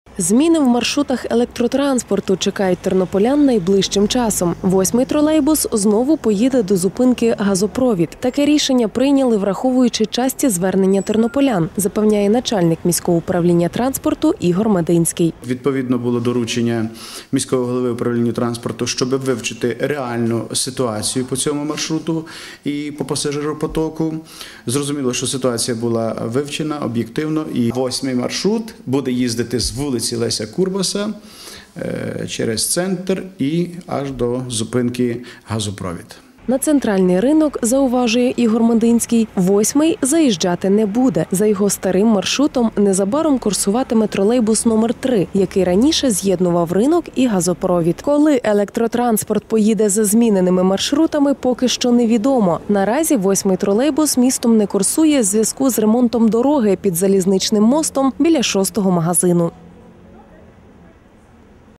Тернопільські тролейбуси за номерами 8 і 3 їздитимуть по-новому
Зміни в маршрутах електротранспорту чекають тернополян найближчим часом. 8-й тролейбус знову поїде до зупинки «Газопровід». Таке рішення прийняли, враховуючи часті звернення тернополян, запевняє начальник міського управління транспорту Ігор Мединський.